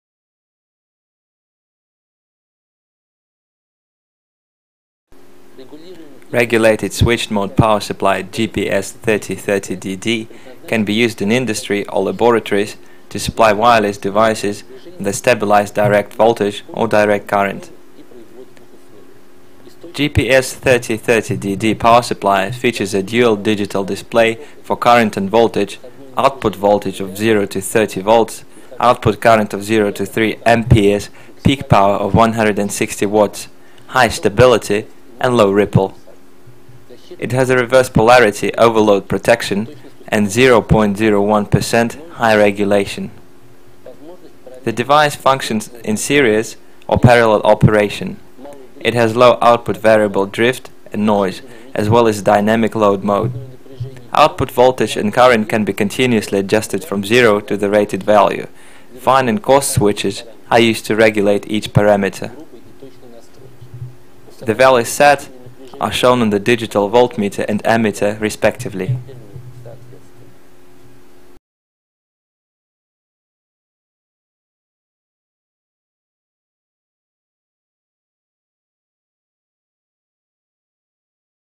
GPS-3030DD Switched Mode Power Supply
GPS-3030DD Switched Mode Power SupplyRegulated switched mode power supply GPS-3030DD can be used in industry or laboratories to supply wireless devices with a stabilized direct voltage or DC. GPS-3030DD power supply features a dual digital display for current and voltage, output voltage of 0 to 30 V, output current of 0 to 3 A, peak power of 160 W, high stability, and low ripple.It has a reverse polarity, overload protection and 0.01% high regulation. The device functions in series or parallel operation. It also has low output variable drift and noise as well as a dynamic load mode.Output voltage and current can be continuously adjusted from 0 to the rated value. Fine and coarse switches are used to regulate each parameter. The values set are shown on the digital voltmeter and ammeter respectively.